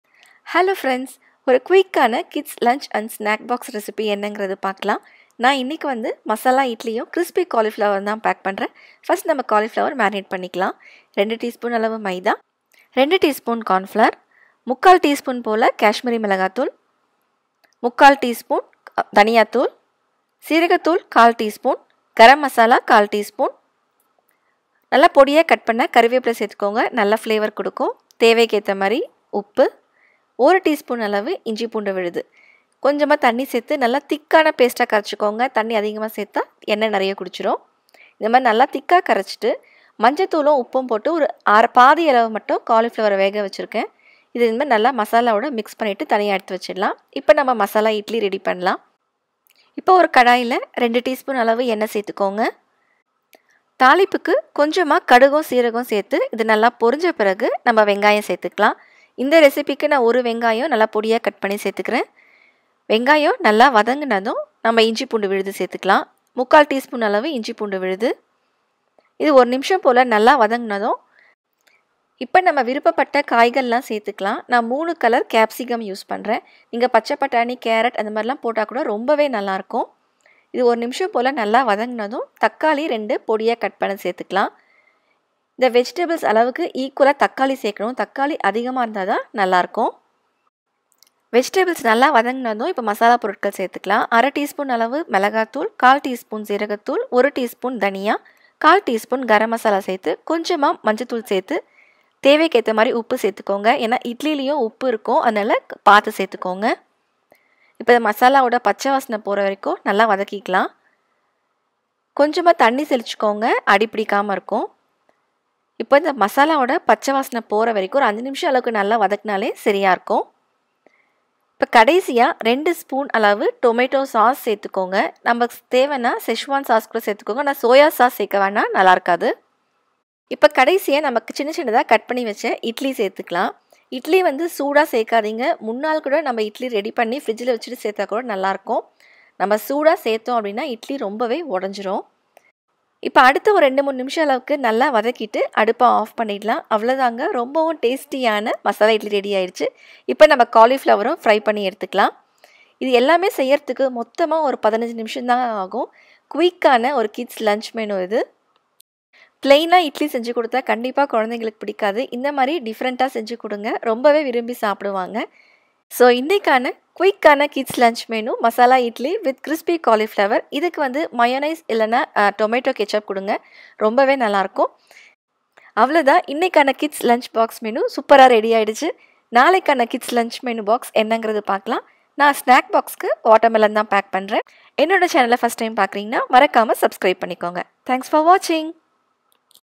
KIDS FAVOURITE LUNCH & SNACK BOX | Masala Idli | Quick lunch menu
Hi friends please check kids lunch and snack box menu.

Please try this at your home and provide me the feedback